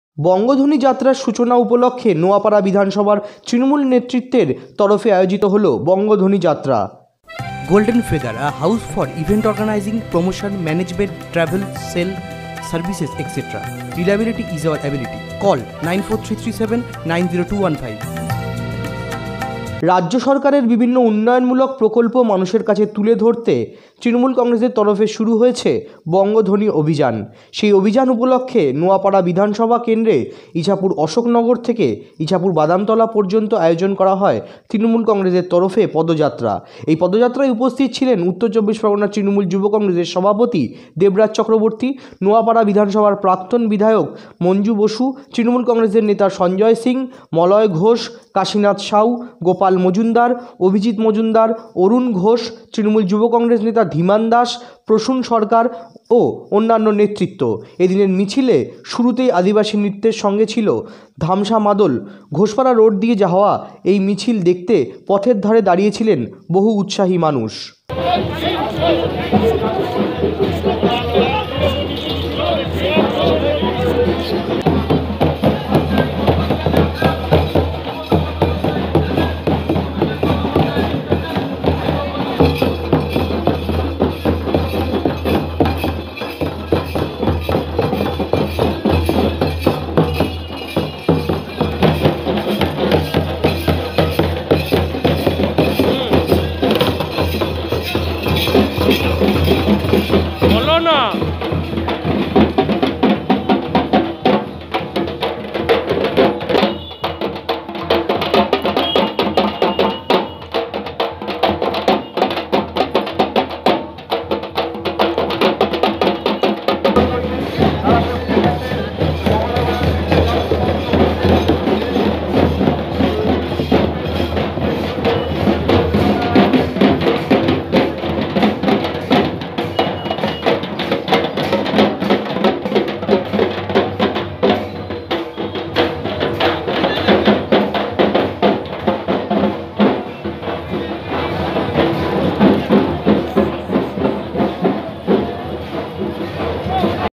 বঙ্গ ধনী যাত্রার সূচনা উপলক্ষে নোয়াপাড়ার বিধানসভার তৃণমূল নেতৃত্বের তরফে আয়োজিত বঙ্গ ধনী পদযাত্রা
বঙ্গ ধনী যাত্রার সূচনা উপলক্ষে নোয়াপাড়ার বিধানসভার তৃণমূল নেতৃত্বের তরফে আয়োজিত হলো বঙ্গ ধনী পদযাত্রা